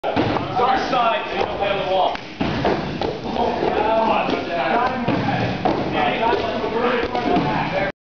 Vaults3
Traceurs performing a few vaults over a table at a parkour session on monday 9-22-08 in the wrestling room.